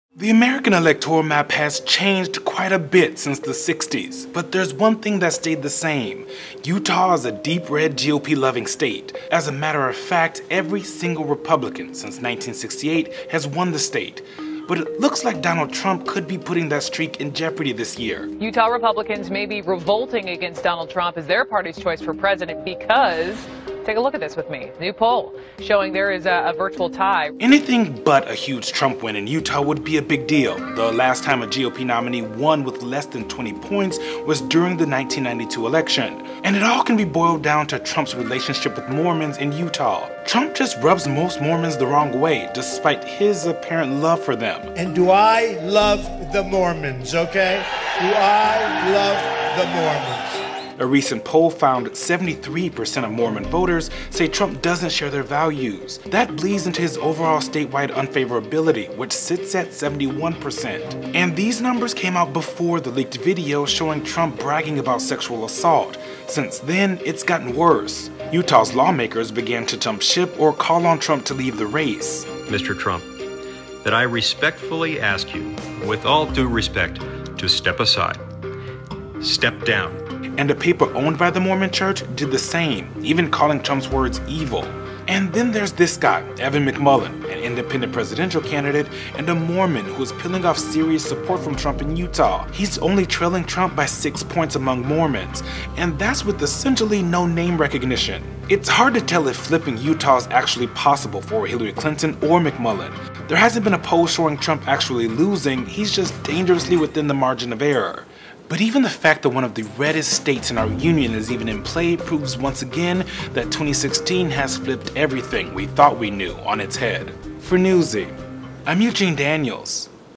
Utah could change colors come November
And it's because Trump's popularity with Mormons is abysmal.